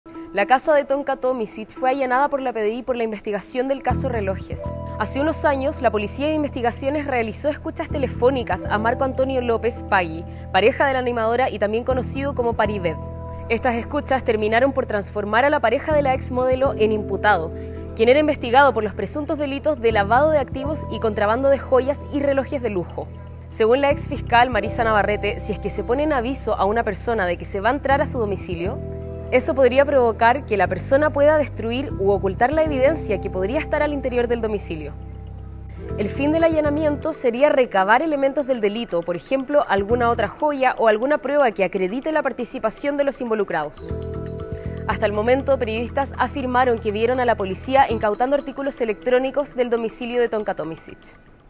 PDI allana casa de Tonka Tomicic: ¿Qué busca la policía?
Durante este martes, un allanamiento se llevó a cabo en el domicilio de la presentadora de TV, Tonka Tomicic, y Marco Antonio López Spagui, su pareja, más conocido como Parived, todo esto en el marco del Caso relojes VIP.

La investigación es a propósito de la adquisición joyas y relojes de lujo que fueron robados y, posteriormente, comercializados.

*NOTAS RELACIONADAS*
📌Caso relojes VIP: Órdenes de detención contemplan a personas que viven desde Ovalle hasta Estados Unidos e Italia